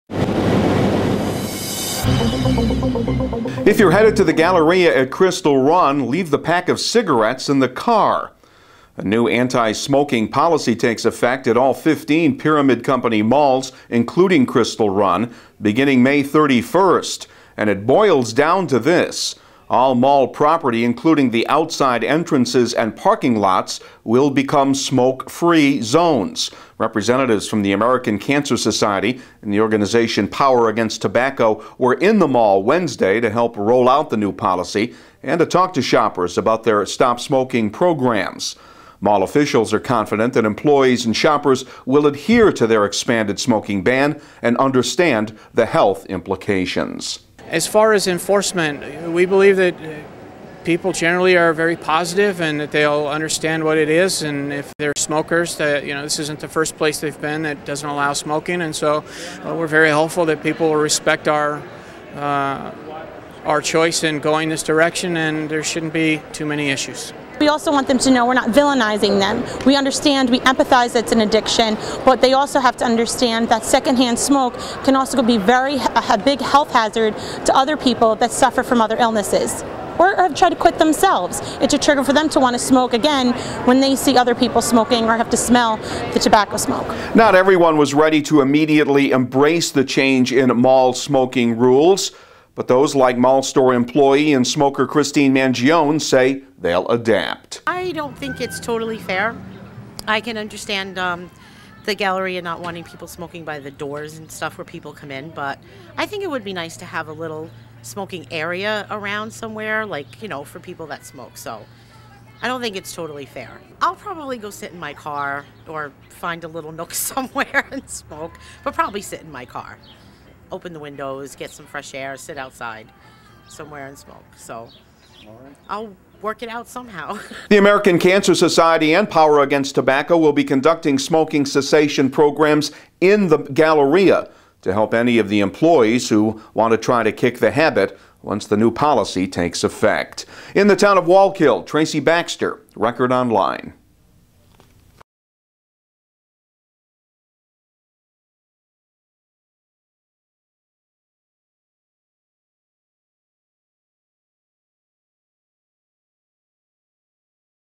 Mall Tobacco-Free Policy
Officials at the Crystal Run Galleria along with the American Cancer Society and POW'R Against Tobacco roll out an expanded anti-smoking policy at the mall.